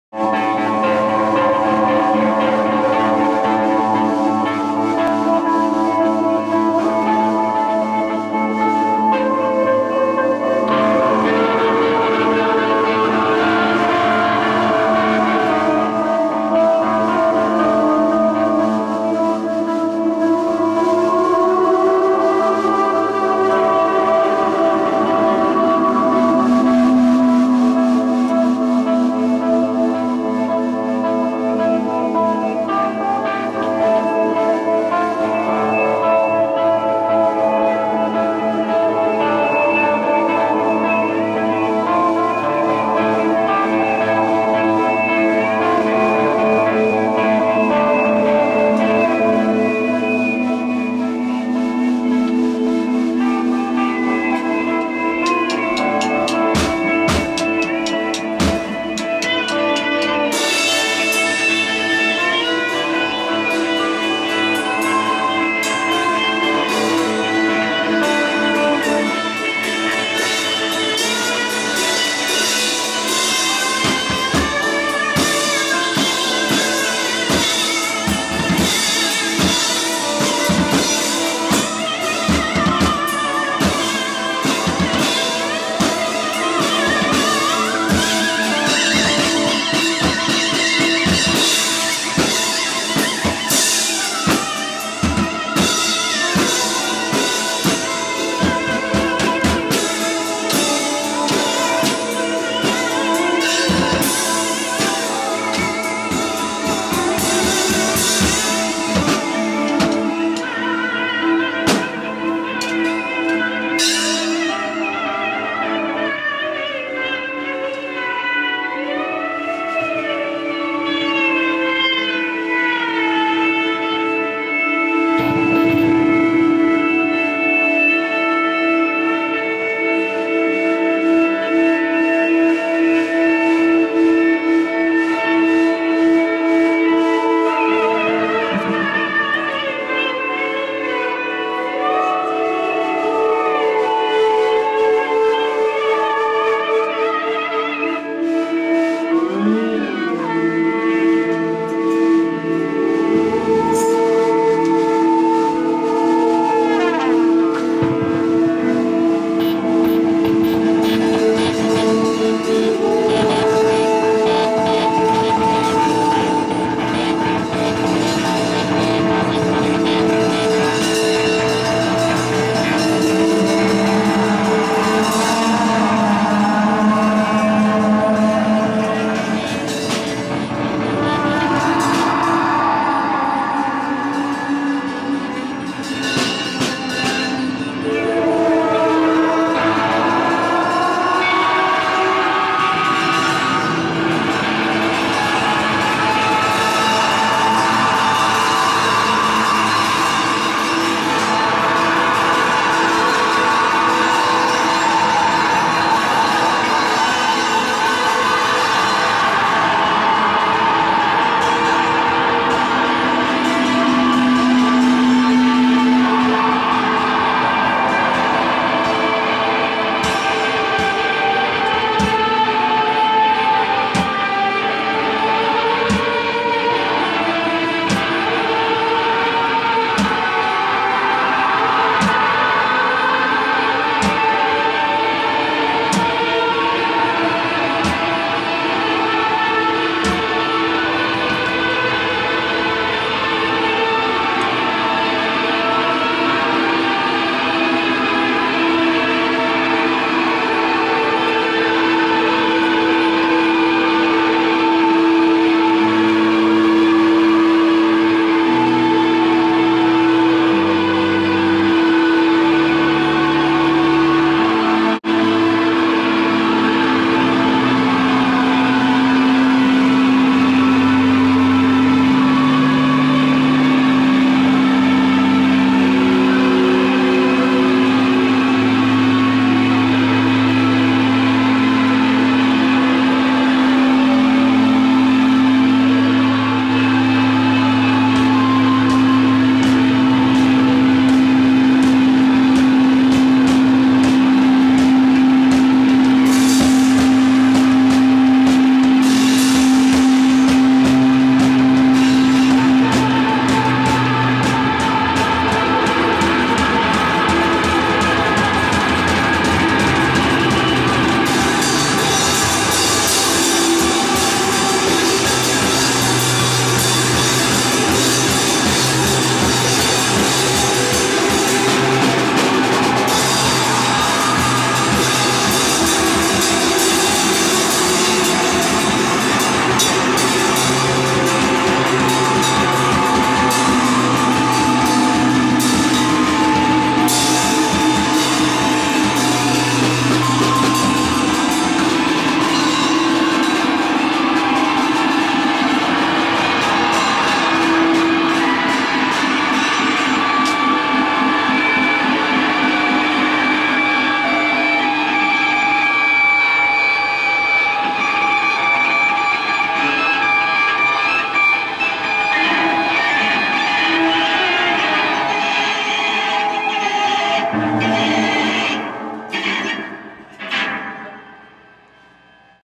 Thee' Electric Chairs @ The Conventicle
Show #4 [Painted Tapes vs. Soothing Almonds Collective] at The Conventicle in Minneapolis (8-17-10)